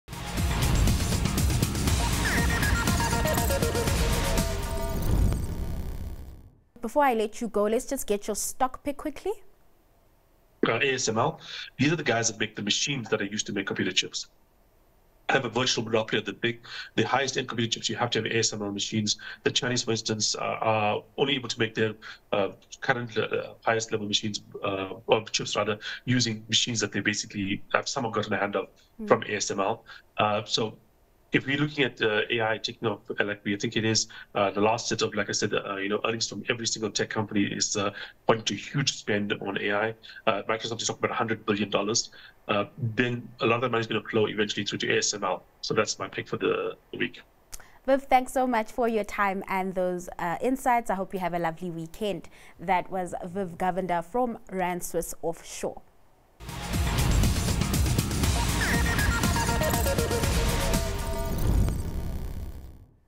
WATCH: Daily Pick - ASML
Viv Govender from Rand Swiss Offshore chose ASML as his stock pick of the day